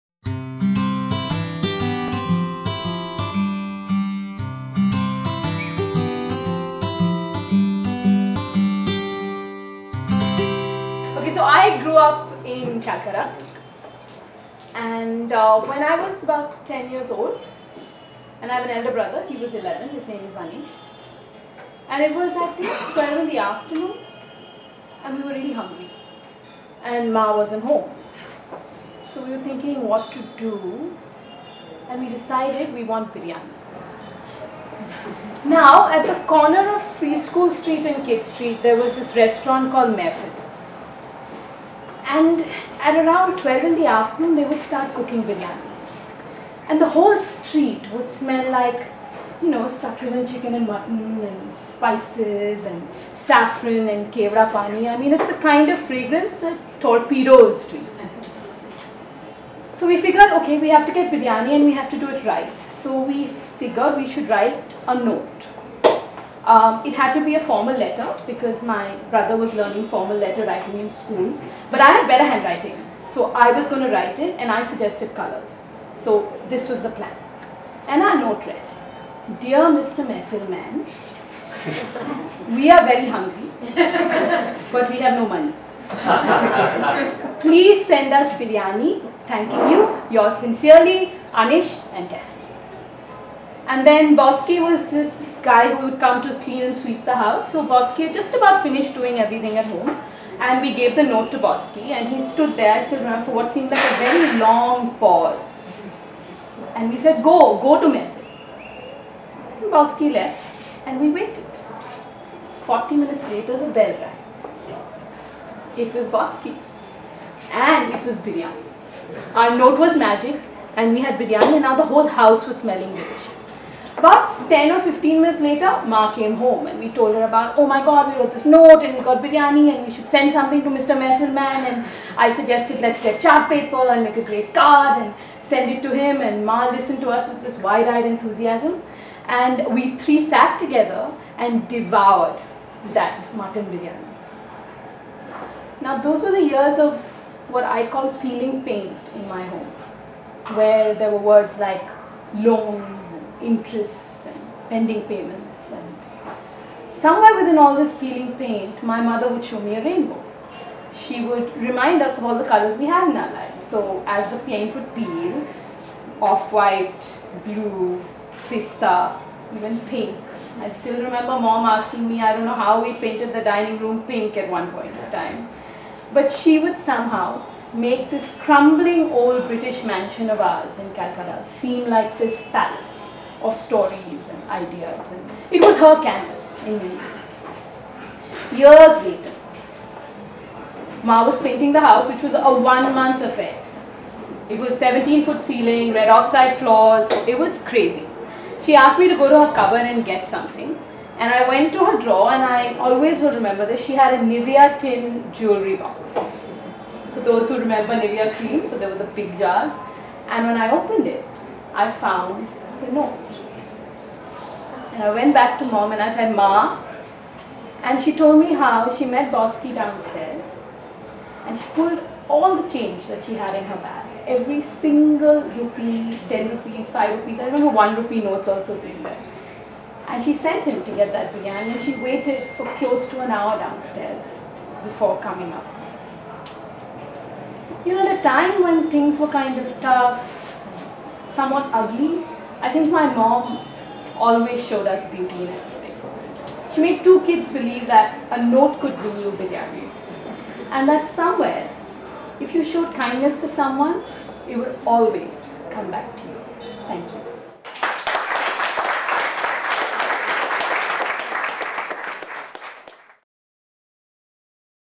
The Biryani Affair - Tess Joseph | The Storytellers
Here's to all the mothers, who've showered us with love and affection, and inspired our keen imagination. Tess' story is a recipe with a dash of nostalgia, a dollop of mother's love and garnished with loads of emotion!

Kommune is a performance arts collective that looks to bring you the best of performing arts.